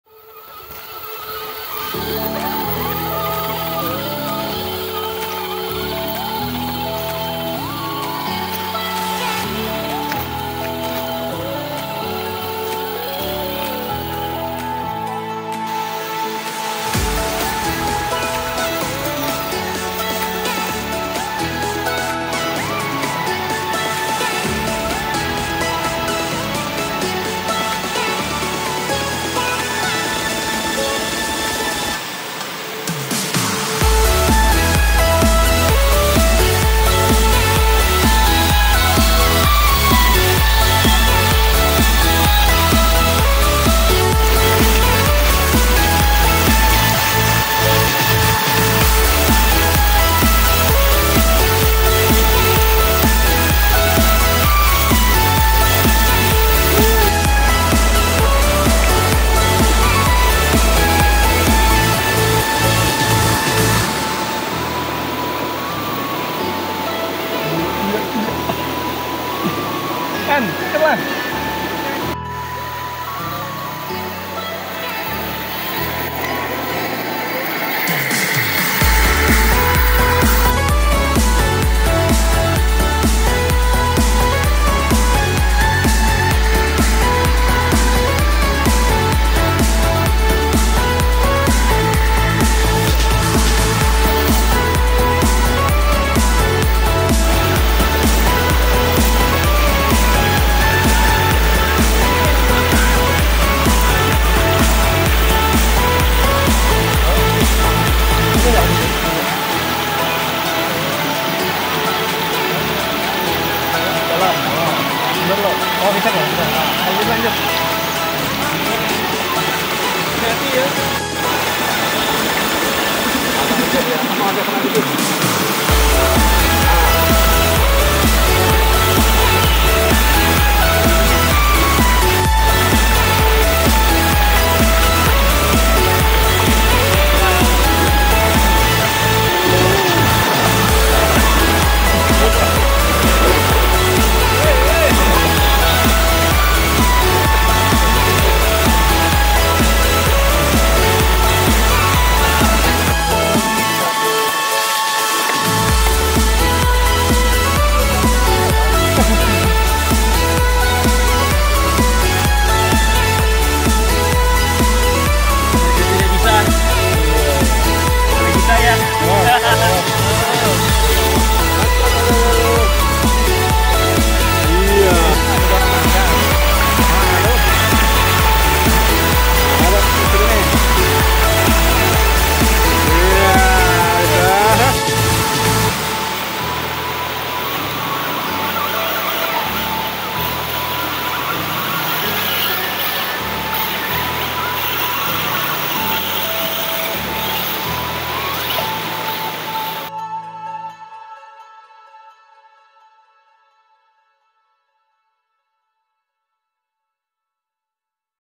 Mabar Riam Ensiling
Berpetualang di alam terbuka memang paling menyenangkan,bersama teman-teman RC Adventure Pontianak.